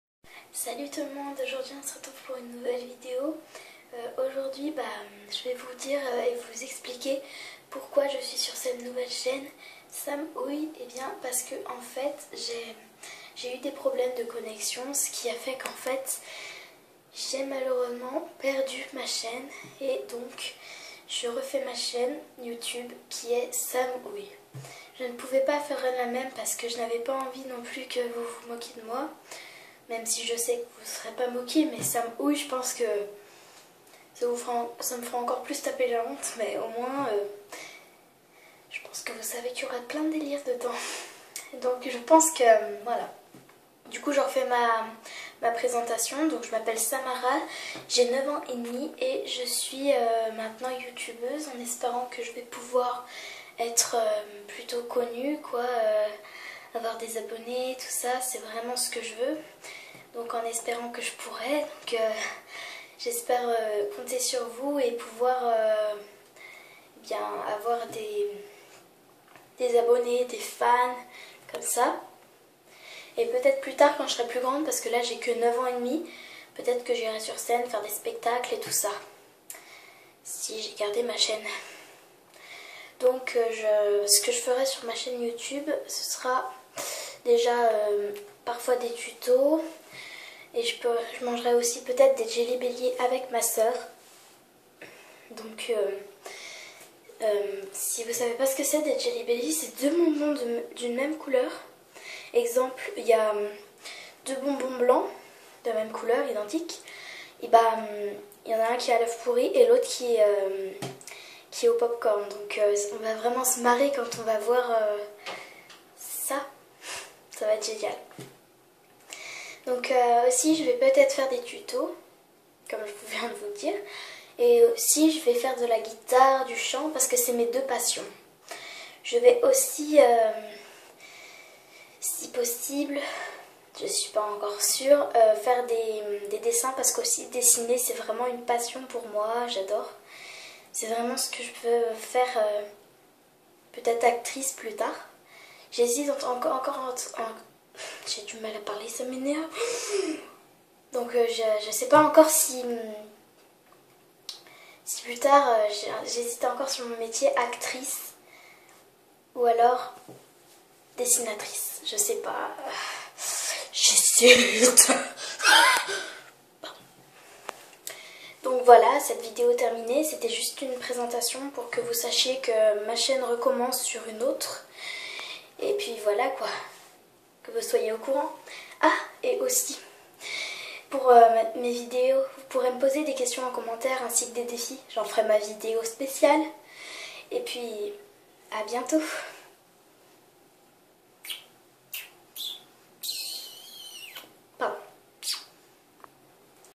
Présentation de mes nouvelle chaîne: Sam Ouille
Salut !!
Il semblerait que ma chaîne Samarah Love est lâchée et conclue de devenir: Sam Ouille...😭😪BIENVENUE  dans mon univer ! Je me présente dans cette vidéo pour ceux qui ne me connaissent pas.
Bisous
                        Sam Ouille